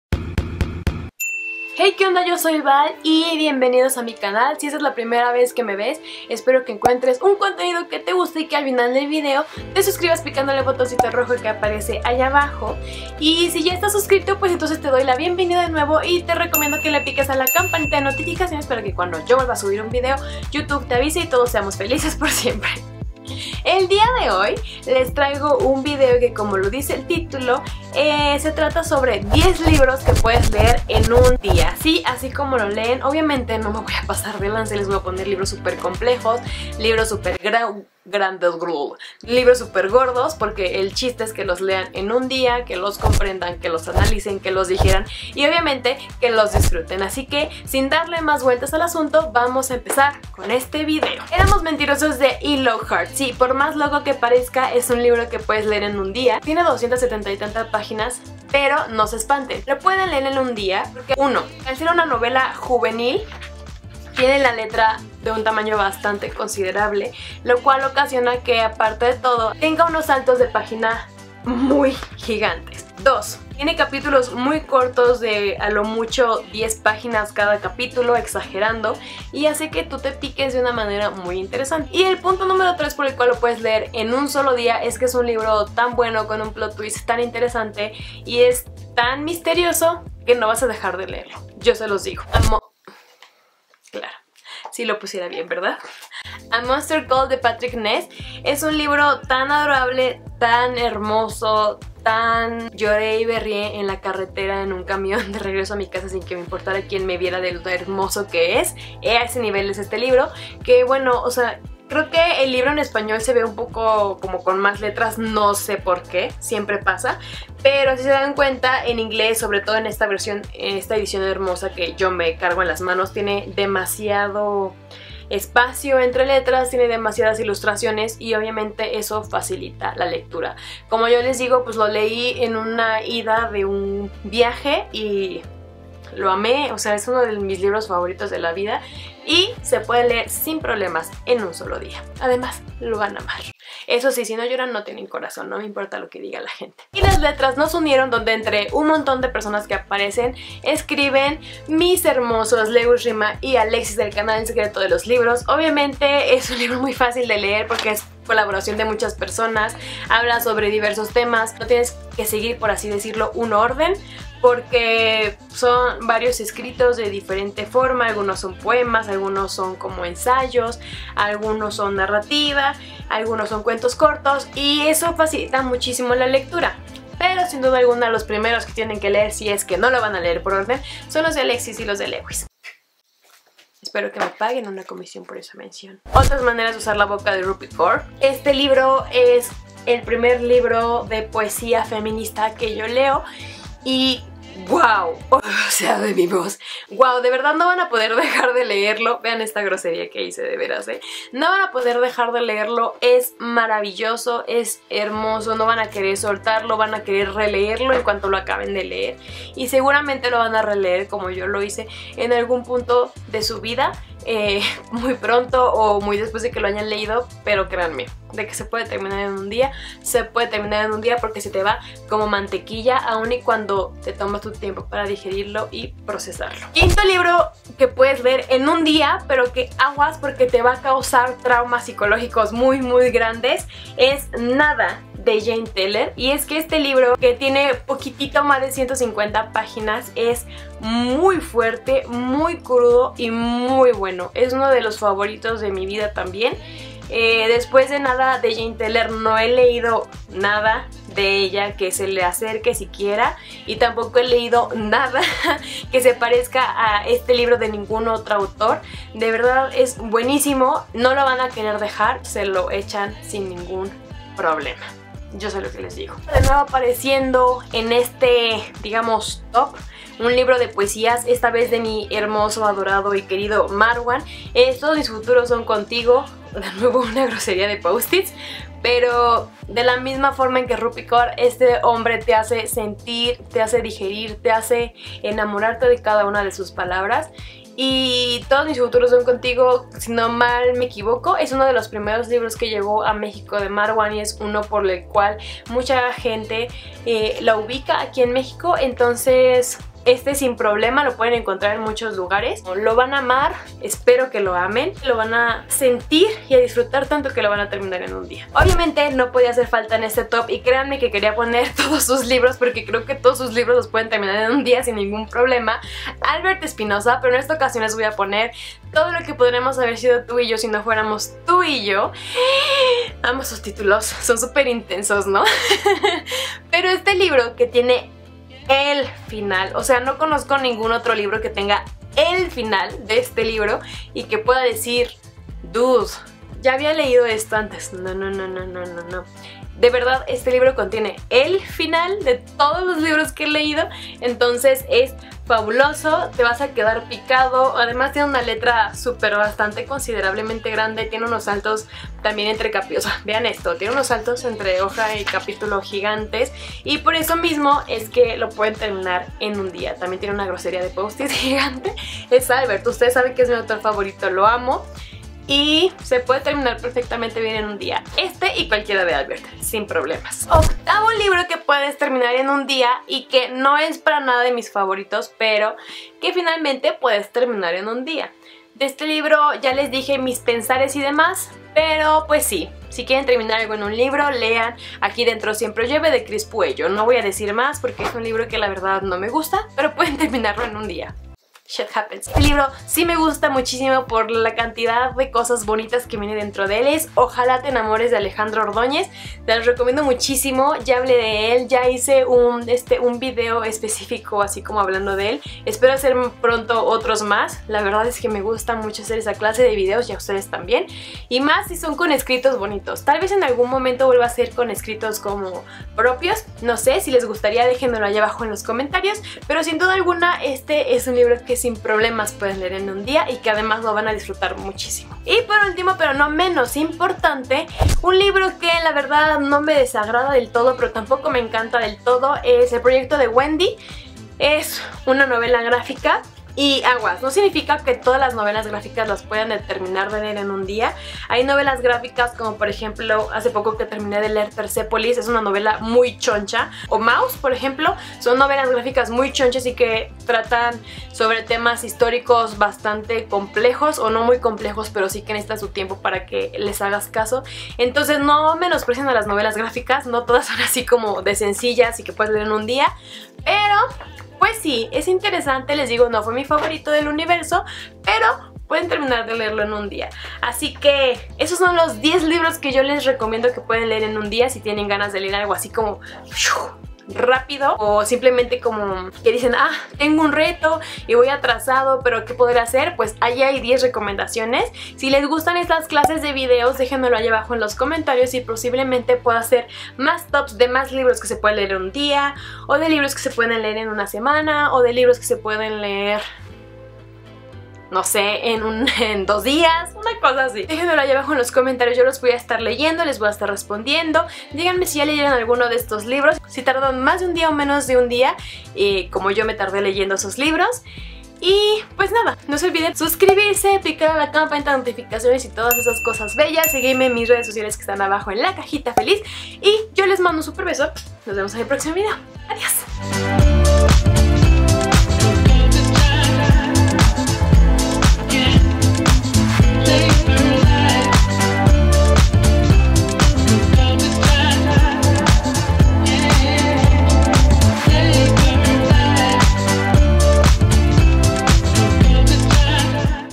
10 LIBROS PARA LEER EN 1 DÍA | Vale Bigotes
¡Soy la CAJITA FELIZ!

¿Tienes algún bloqueo lector o quieres cumplir algún reto de lectura express? Entonces, este video te ayudarán, ya que te doy 10 recomendaciones de libros que podrás leer en ¡1 sólo día!